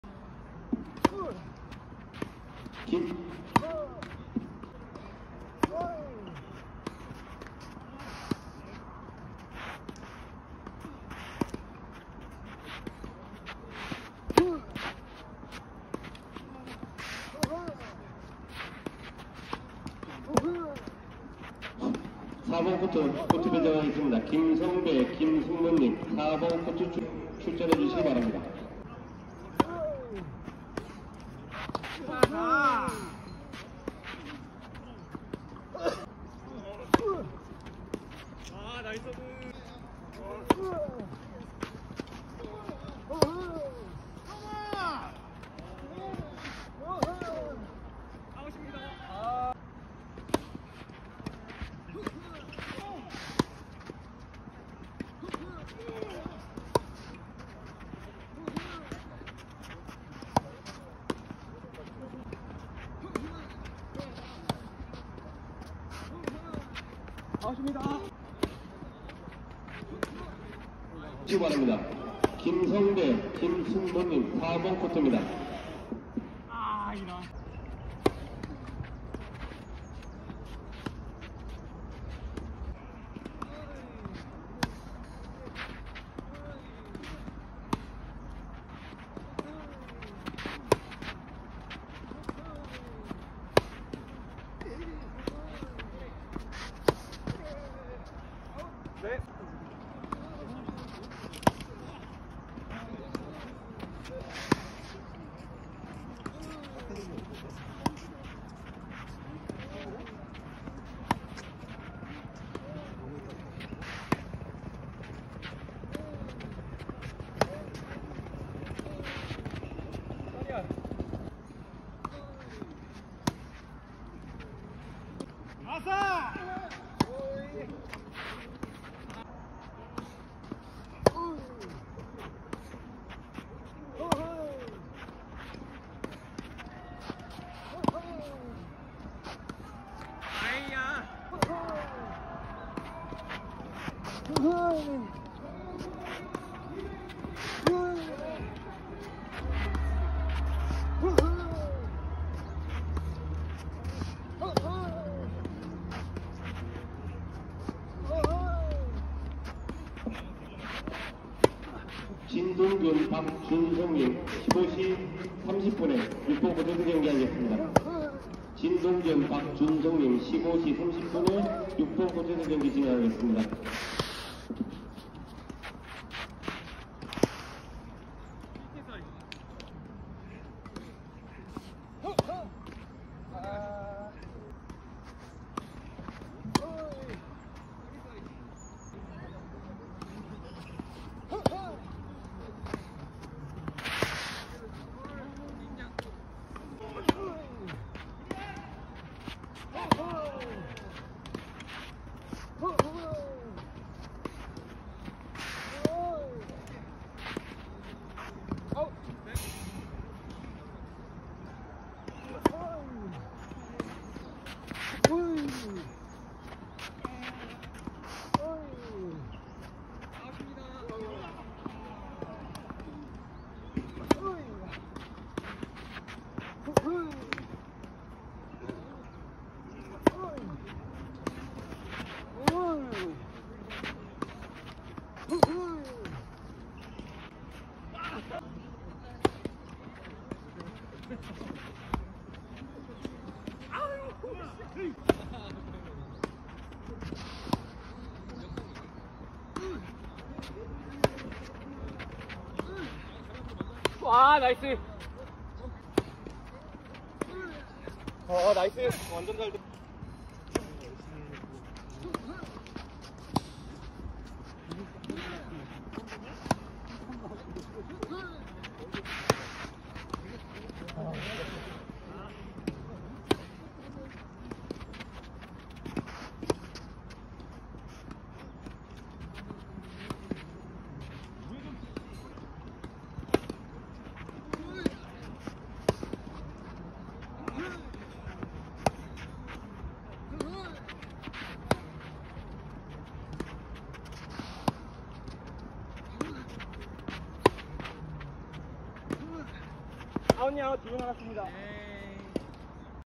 원주치악배 3그룹 테니스 단식대회 16강(김평강 vs 김민규)
원주치악배 16강전 입니다